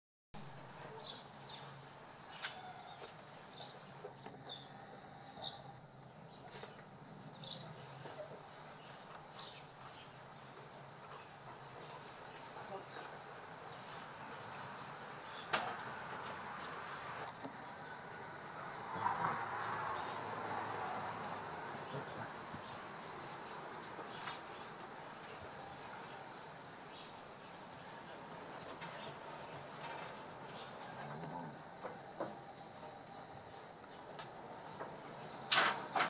Gordon Setter Welpen B-Wurf #10
12.Lebenswoche

Gordon Setter Welpe wird von seiner Mama erzogen